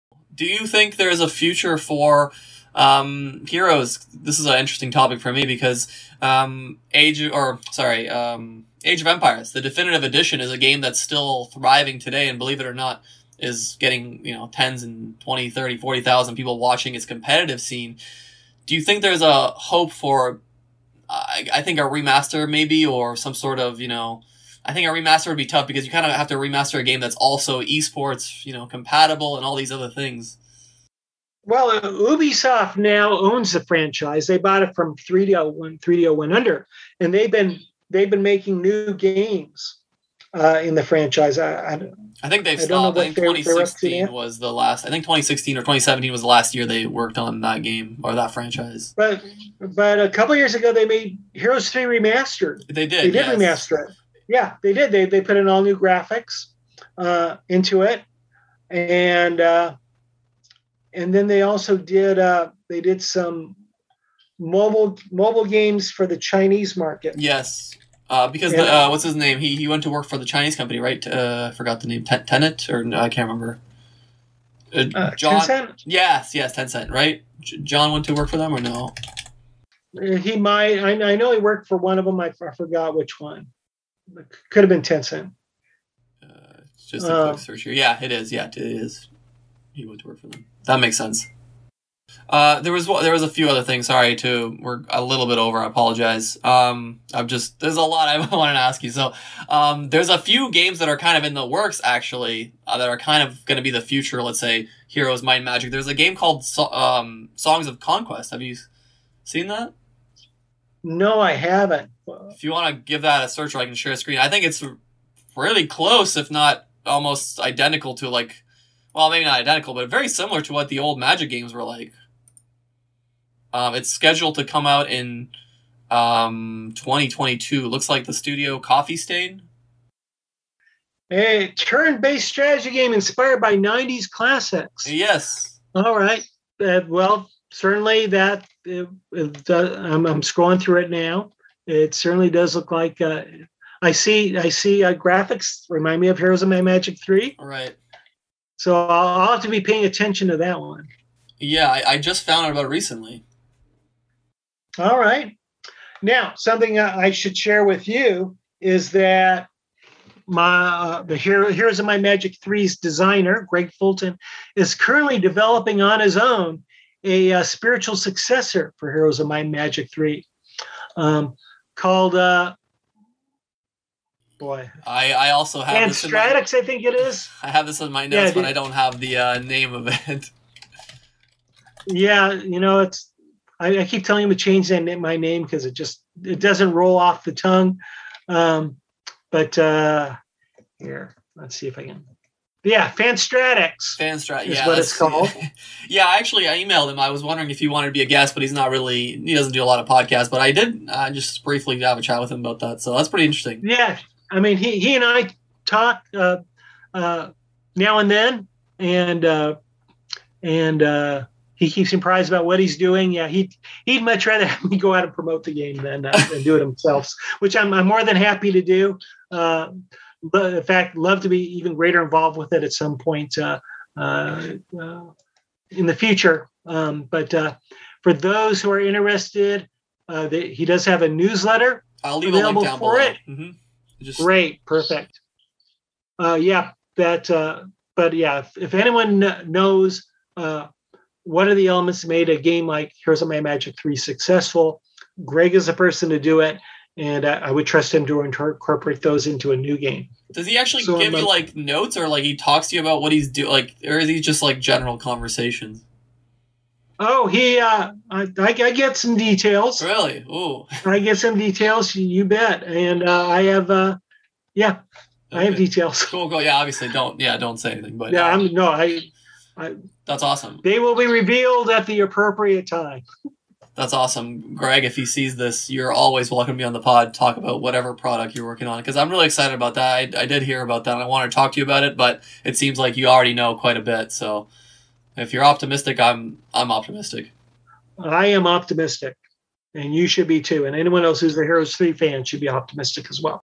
Heroes of Might & Magic Developer Talks About New Games
In this clip David talks about new games in development that are similar to the old Heroes of Might and Magic 3 games.

David Mullich is a world class video game developer with over 25 years of video game design and development experience. He has worked on iconic games such as The Prisoner, I Have No Mouth, and I Must Scream, Disney's Ducktales and the Heros of Might & Magic franchise.